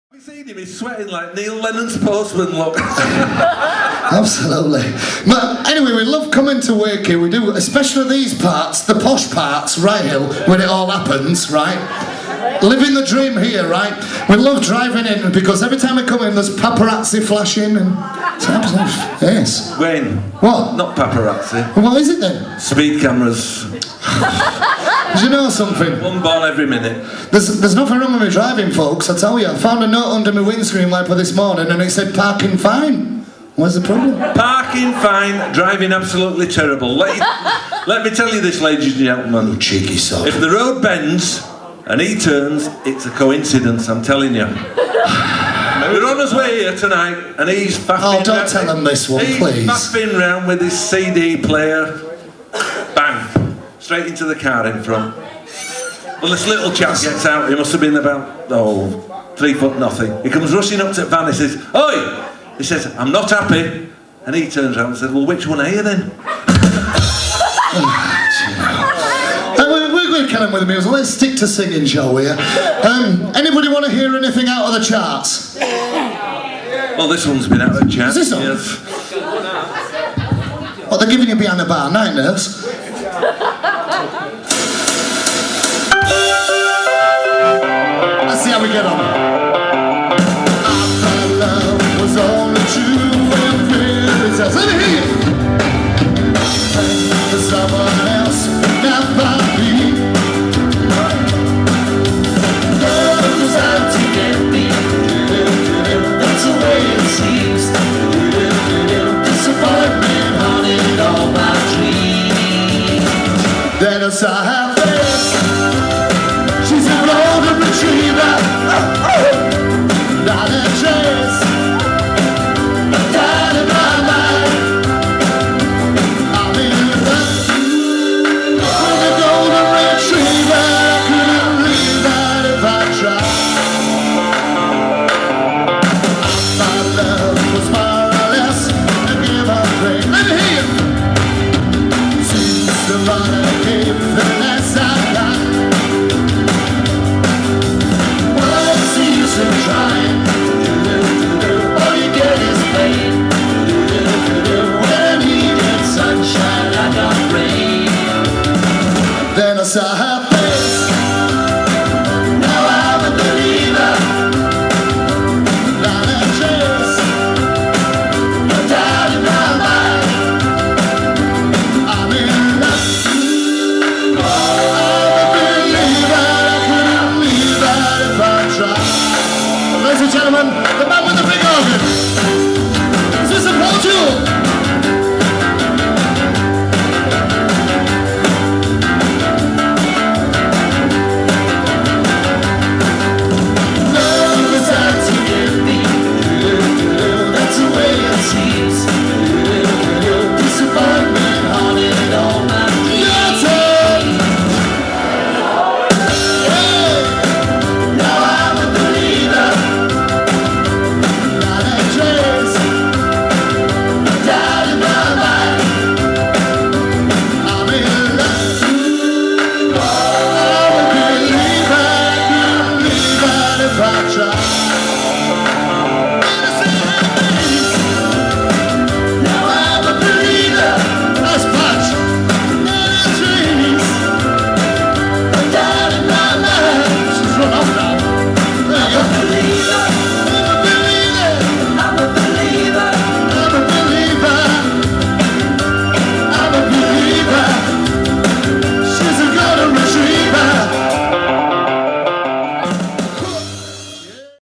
Don't Tell The Wife At Ryhill Liberal Club
Don't Tell The Wife Performing At Ryhill Liberal Club's STAR NIGHT